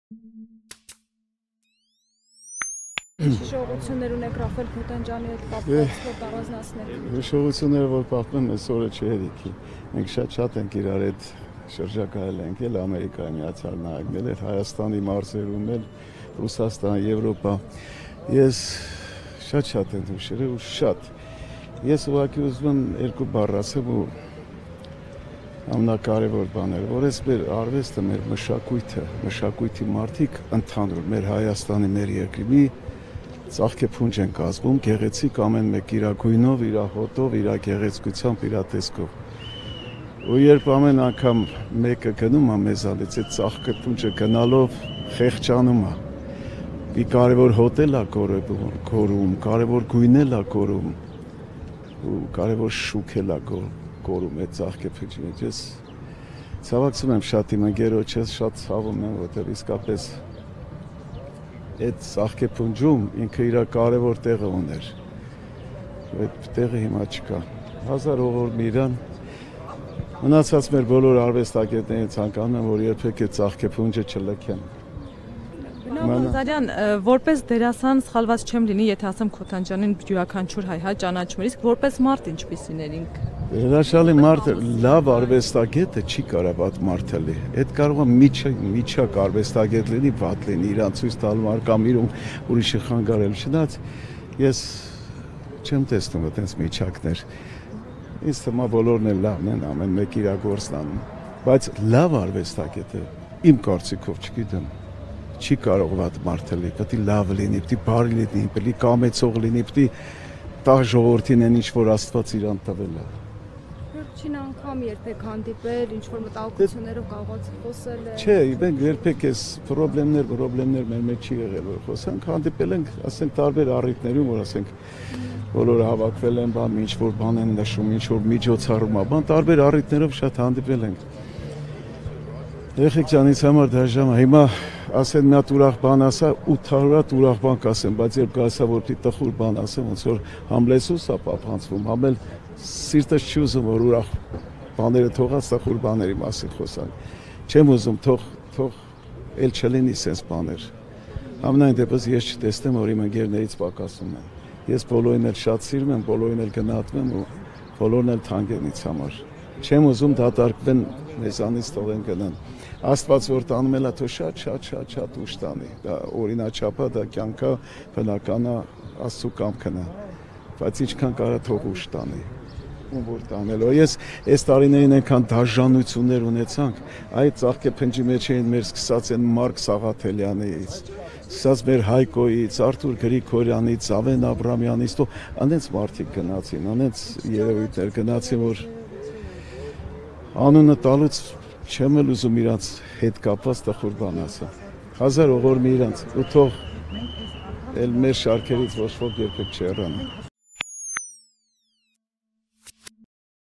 Ռաֆայել Քոթանջյանը հրաշալի արվեստագետ էր, բարի էր ու կամեցող. Աշոտ Ղազարյան
Մեր մշակույթի մեջ Քոթանջյանն իր տեղն ու բույրն ուներ, նրա տեղը լրացնել հնարավոր չի լինի. դերասան